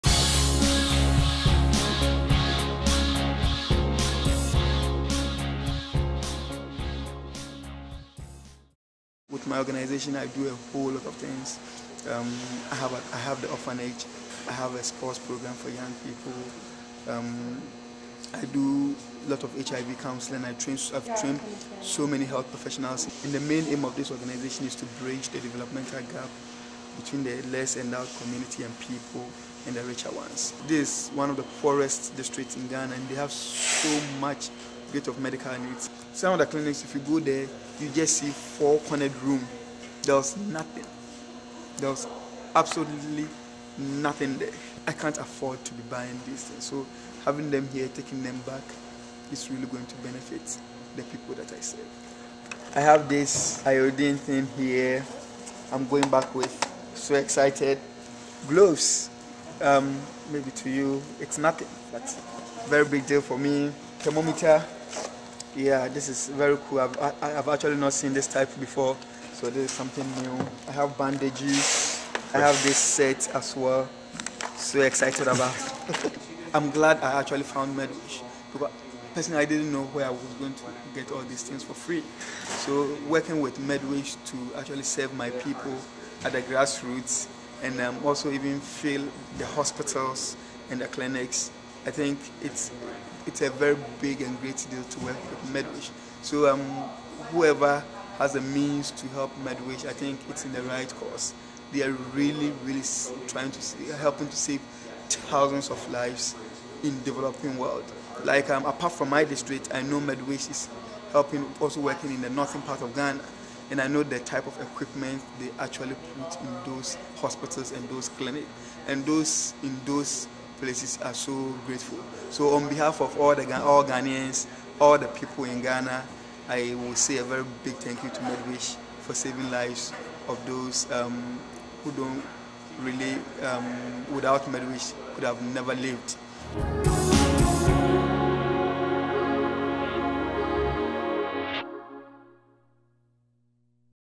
Meet Elvis Domko of Ghana
Elvis Domko runs an orphanage in Ghana. Hear about the supplies that MedWish provided him with, and what they mean to supporting his efforts there.